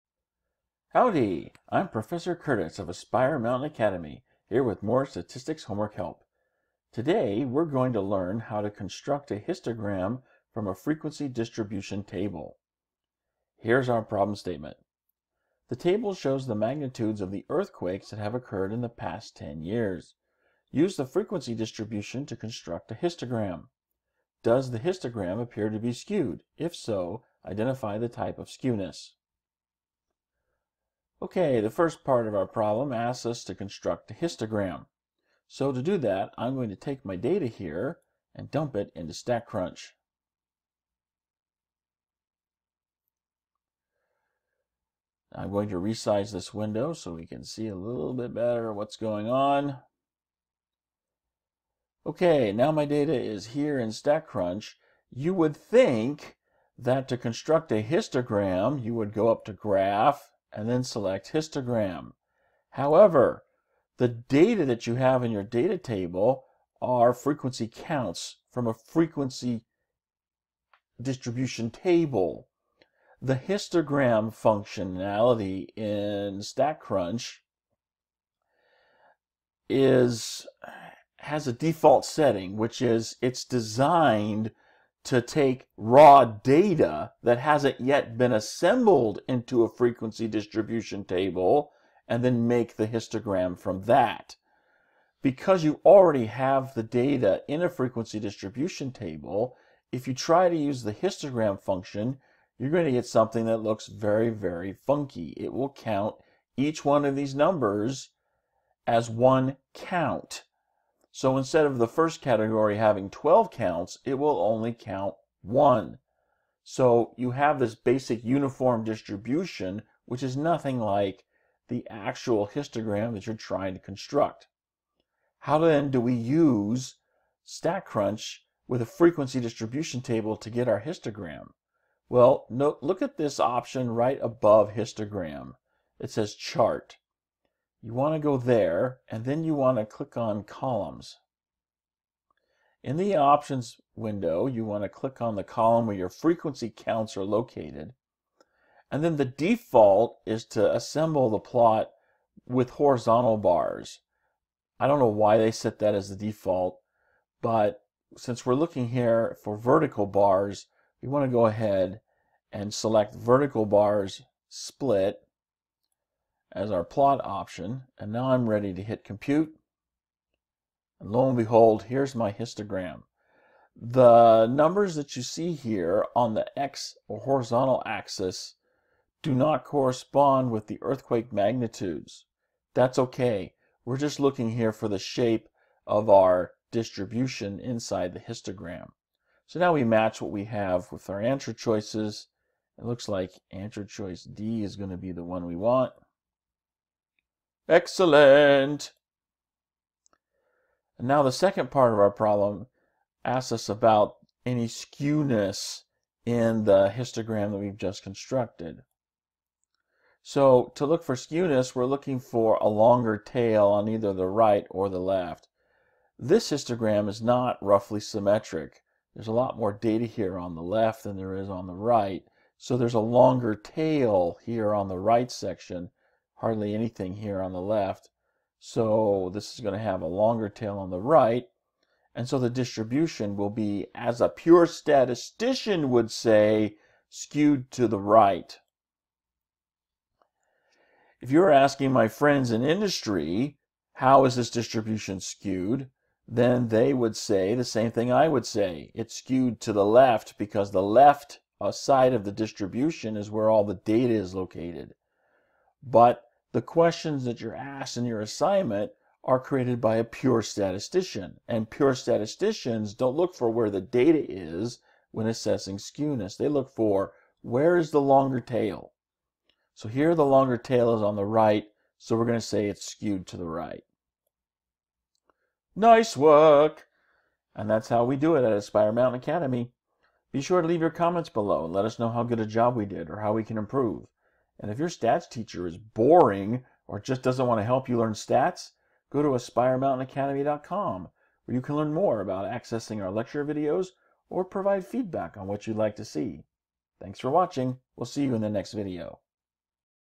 [2.2.13] Constructing a histogram from a frequency distribution table
In this video, Professor Curtis uses StatCrunch to demonstrate how to construct a histogram from a frequency distribution table (MyStatLab ID# 2.2.13).